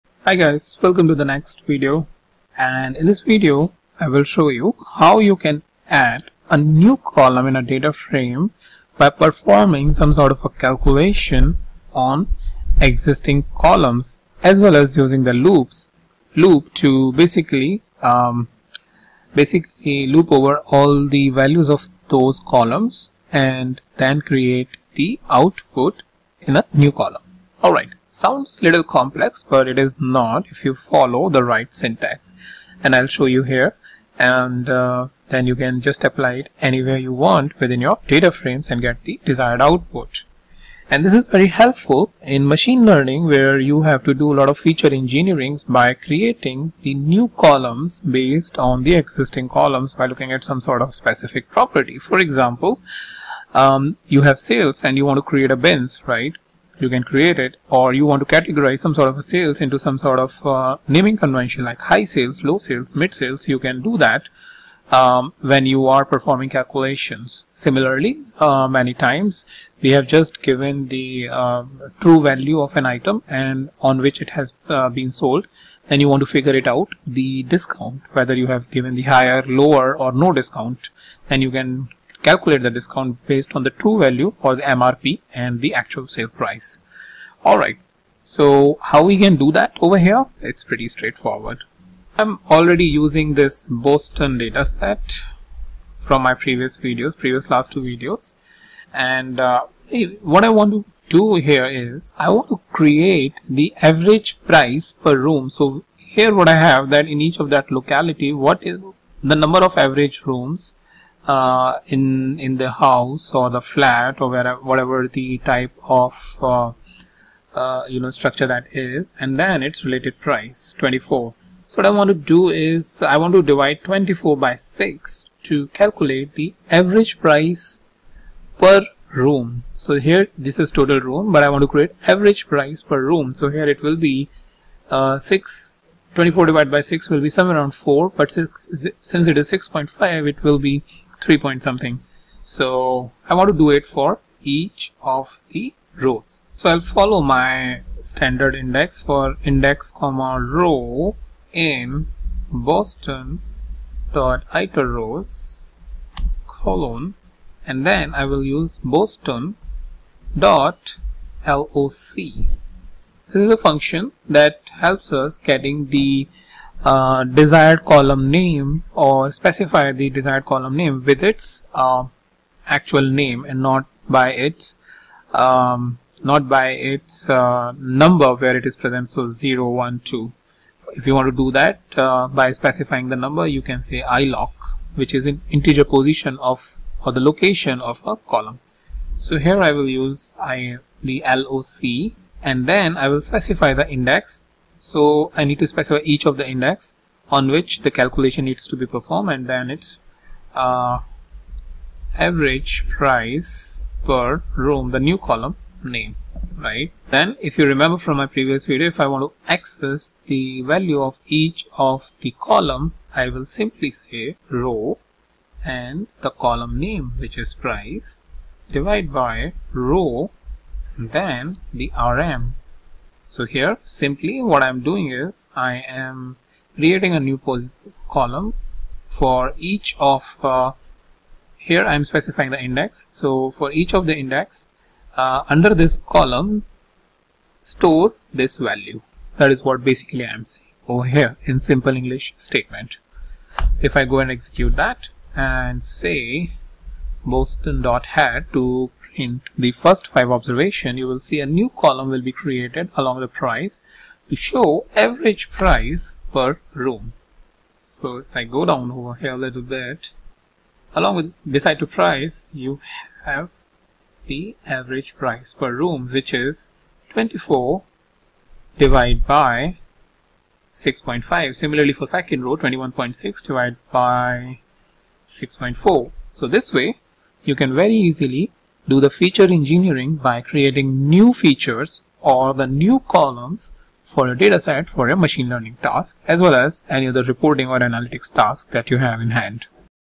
Python 3 Programming Tutorial 13 | Loops | How to loop over dataframe & create new calculated column
In this Python 3 Programming Tutorial 13 video I have talked about  How to loop over dataframe & create new calculated column. This is a very useful functionality all the time you need for data pre processing task.

My Collection of 100 Pandas Tips and Tricks in Jupyter Notebook - You can buy it